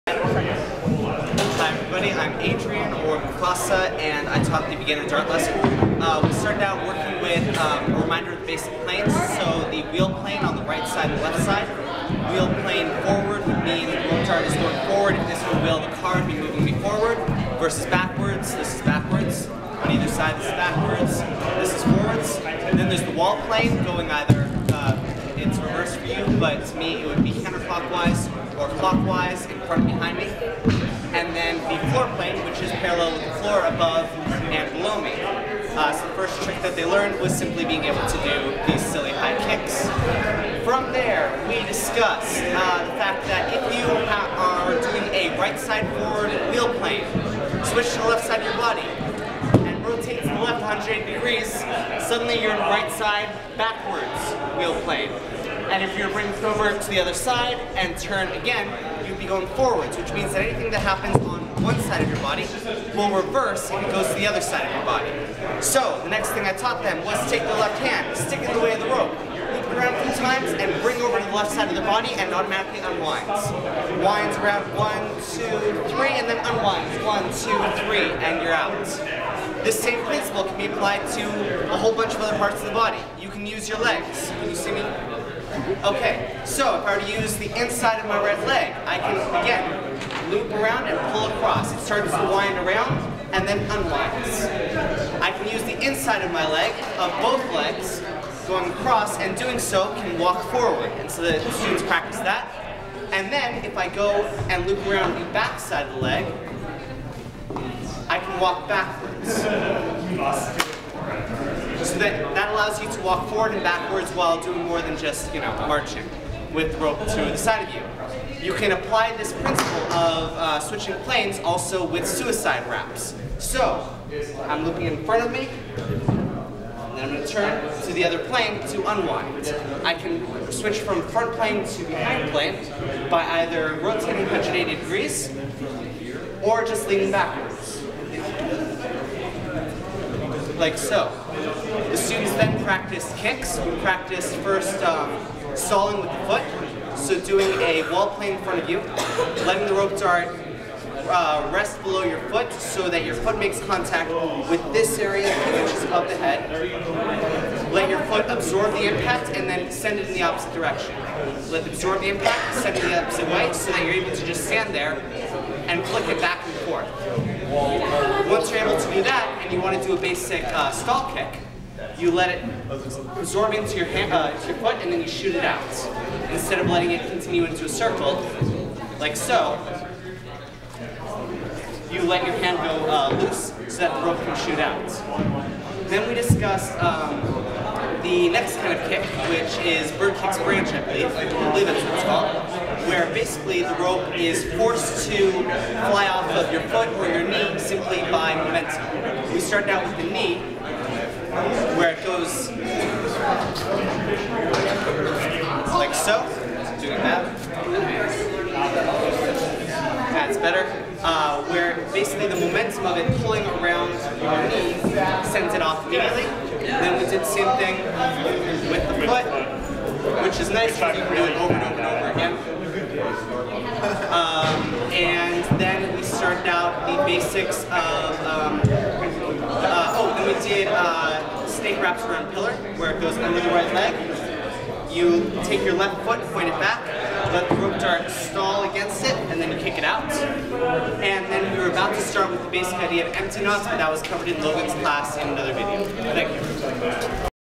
Beginner Rope Dart [WOMBAT February 2012 Class Review Video]
Review video by Adrian (Mufasa), who taught the "Beginner Rope Dart" class at WOMBAT, the Winter Object Manipulation Bootcamp At Tufts, February 2012.